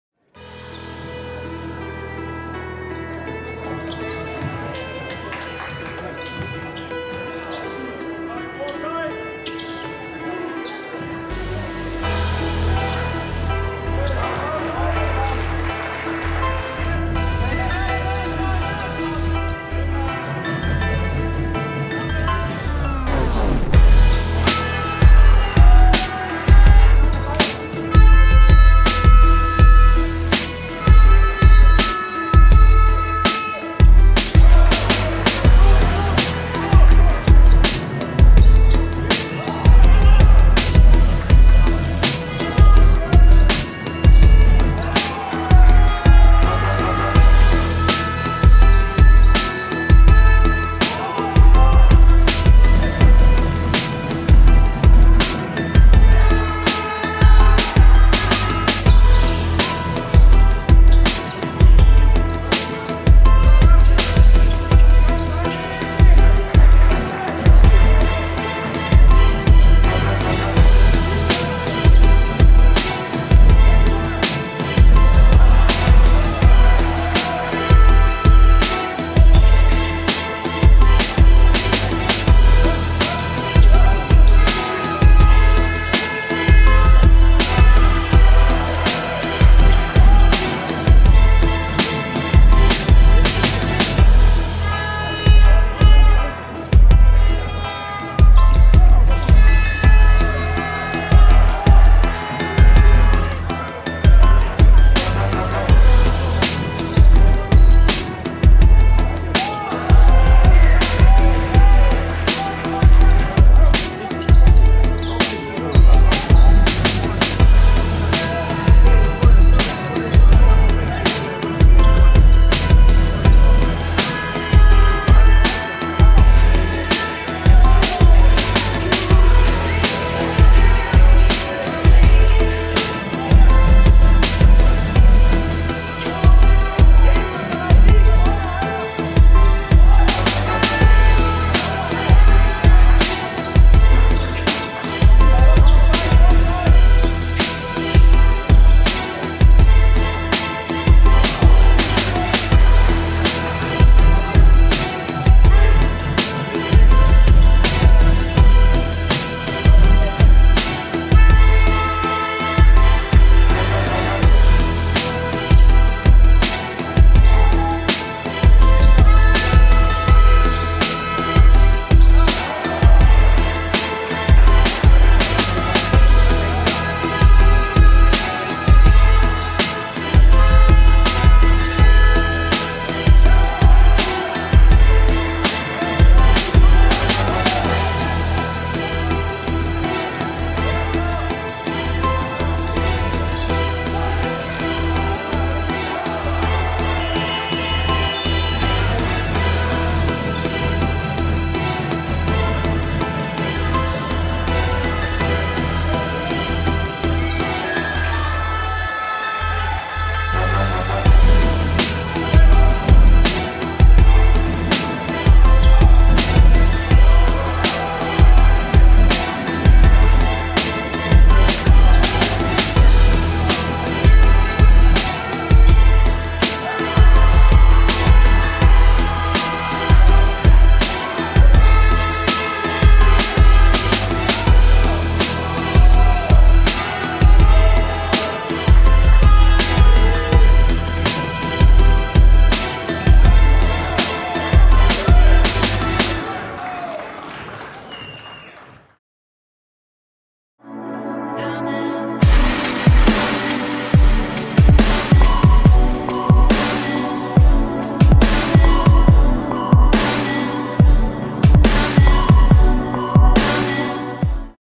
BBL: Kalev/Cramo vs. KK Pärnu 73:83 (2014/15)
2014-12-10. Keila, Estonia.